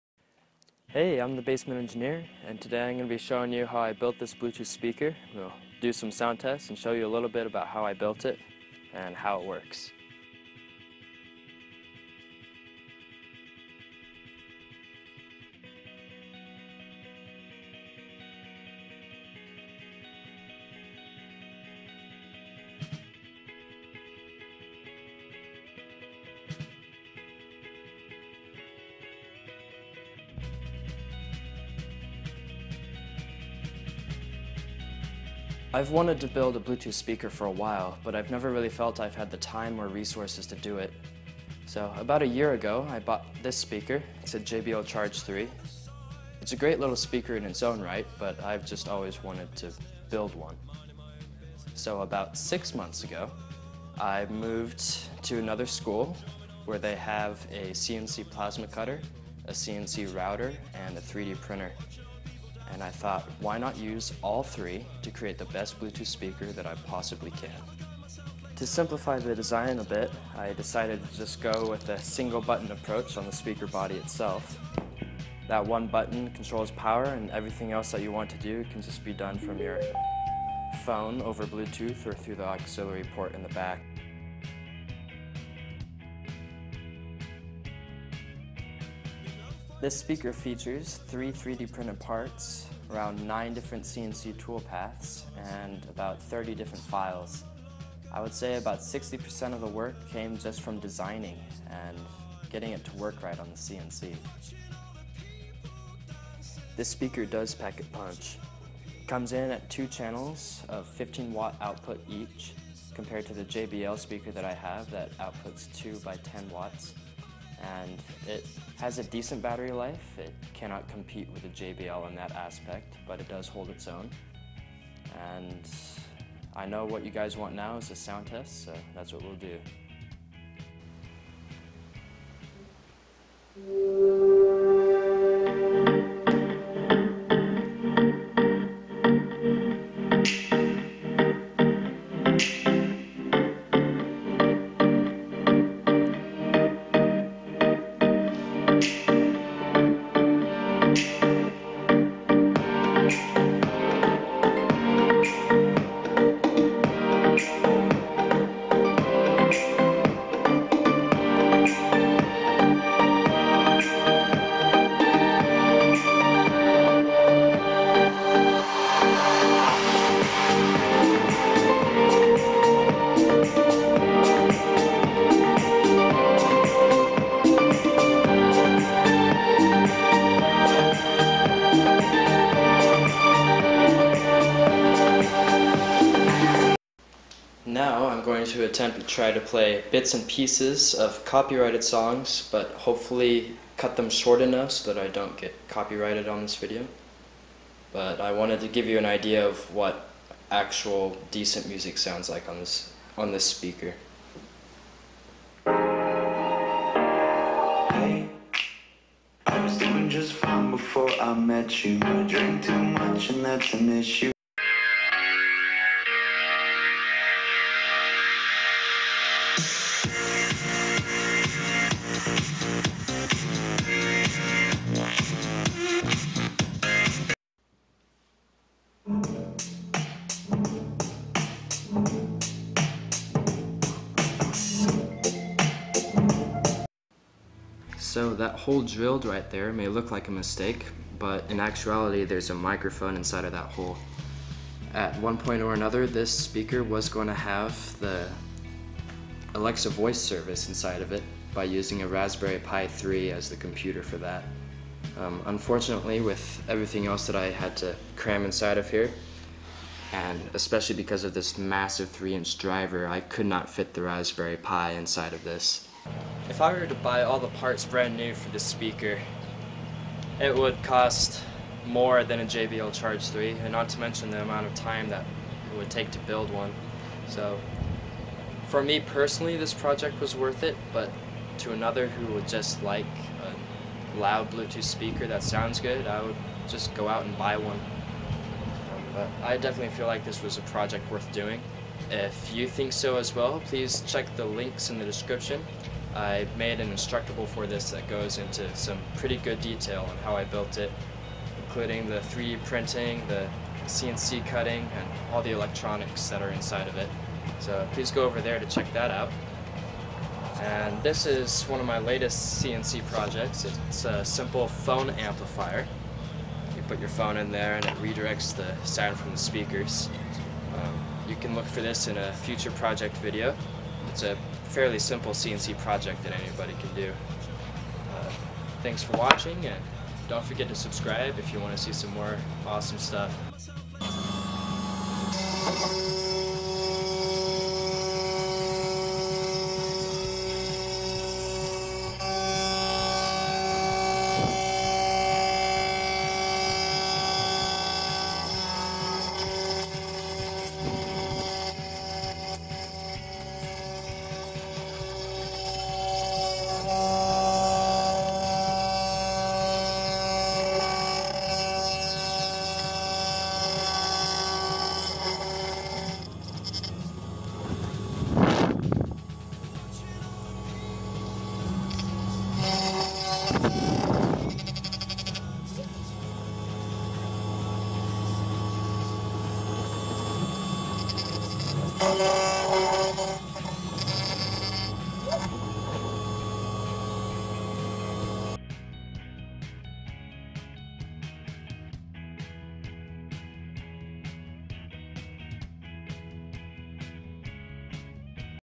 The Ultimate CNC Bluetooth Speaker
This DIY Bluetooth speaker was inspired by the JBL Xtreme. It may not have the same capabilities as the Xtreme, but it was still a fun project.
This was created with a Makerbot Replicator + 3D printer, a Maverick Legacy CNC Router, and a MultiCam 1000 Plasma CNC.
I am a beginner in 3D printing, CNC's, and 3D design in general, but I still thought I could do something pretty cool.
where I go into detail on how I made this.
This is one of three projects coming with me to the Maker Faire in SLC on April 15th. Swing by if you want to see it in person.